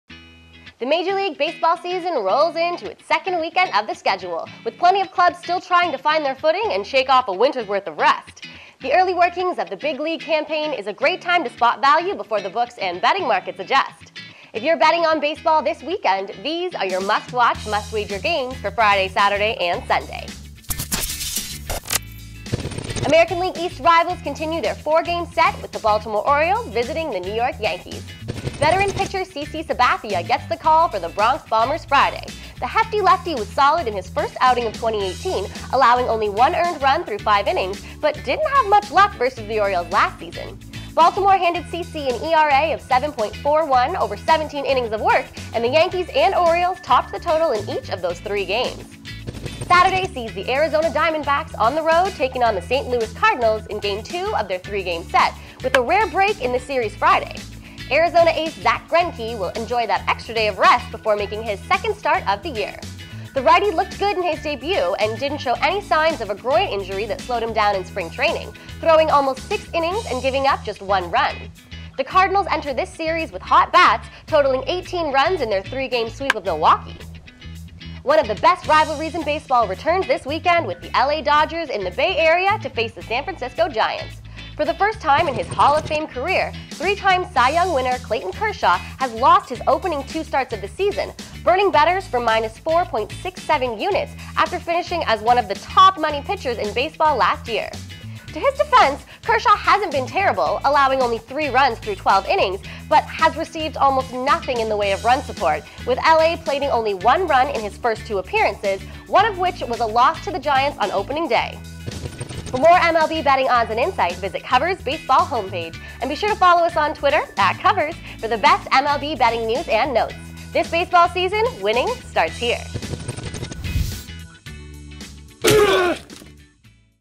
The best must-watch, must-wager games in MLB betting this weekend
The Major League Baseball season rolls into its second weekend of the schedule, with plenty of clubs still trying to find their footing and shake off a winter’s worth of rust. The early workings of the big-league campaign is a great time to spot value before the books and betting markets adjust. And if you’re betting on baseball this weekend, these are your must-watch, must-wager games for Friday, Saturday, and Sunday.